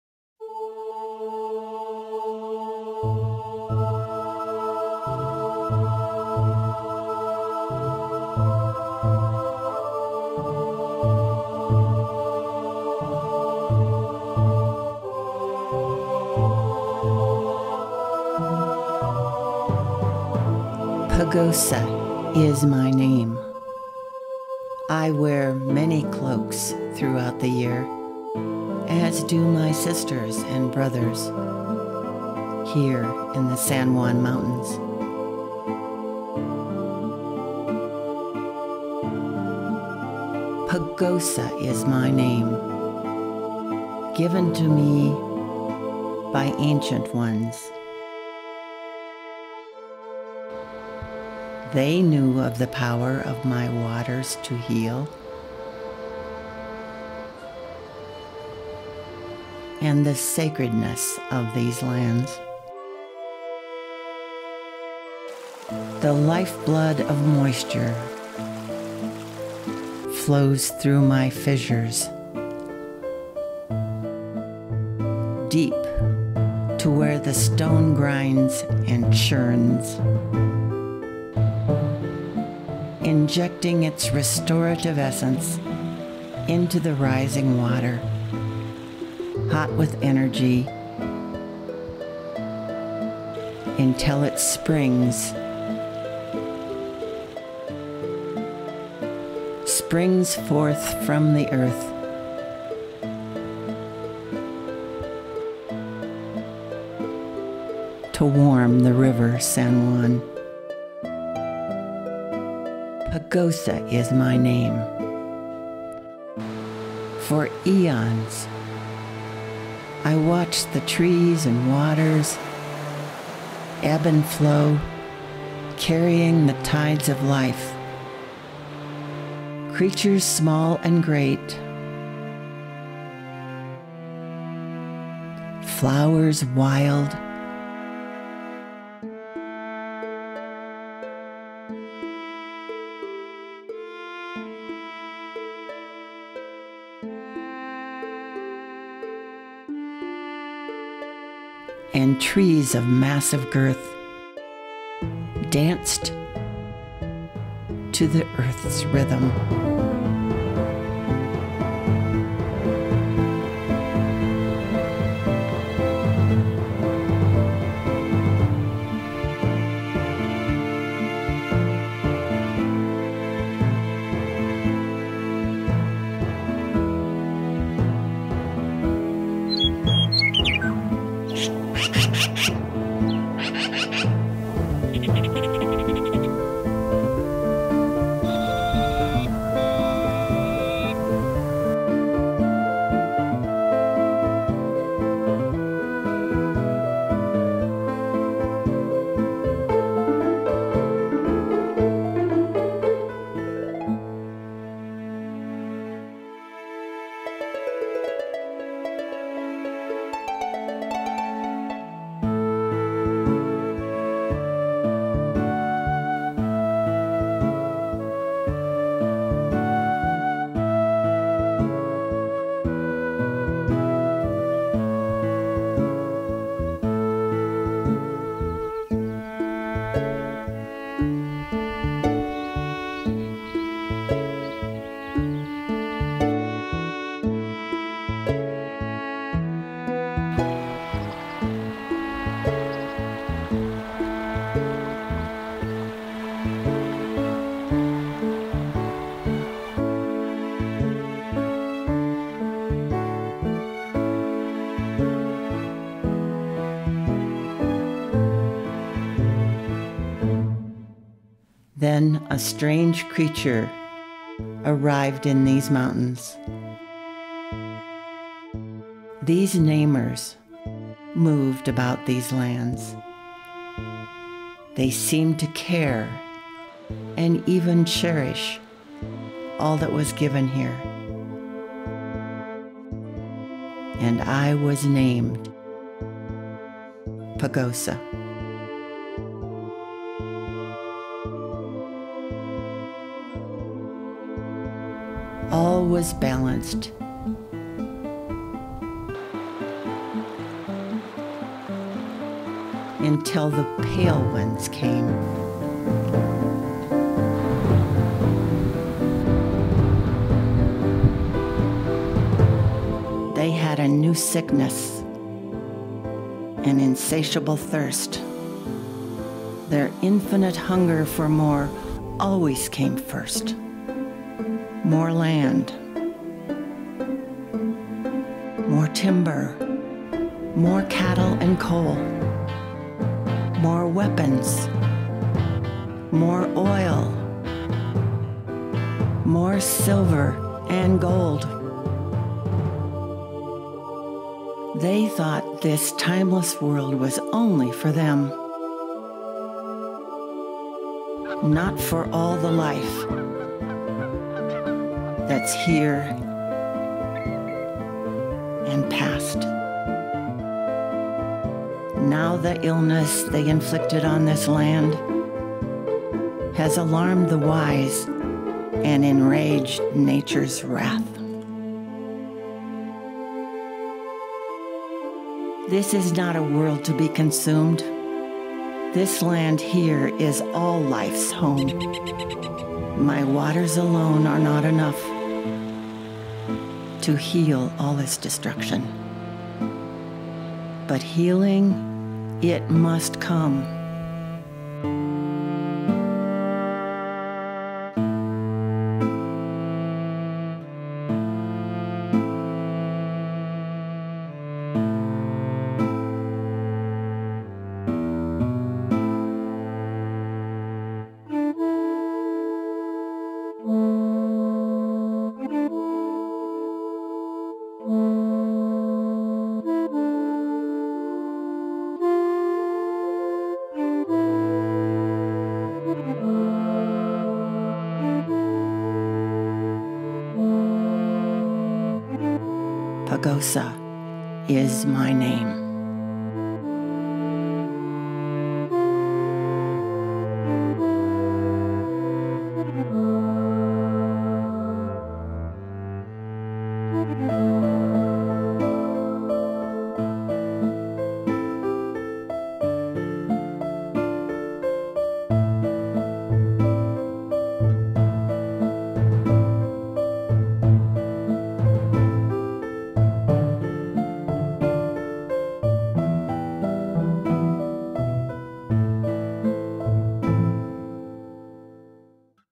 Wildlife Filmmaking - Pagosa Is My Name
As a wildlife filmmaker, there are times when I want to tell a deeper story, one that is more accurately classified as natural history. This is my interpretation of the area where I live through the eyes of the mountains that surround my home, the San Juan Mountains and in particular, Pagosa Peak. My films are a combination of 4k video and stills and in this case the cameras are a Sony RX10 mk4 and a Sony A7r mk4 with the Sony 100-400 G lens.
This film won 15 awards on the festival circuit in 2022.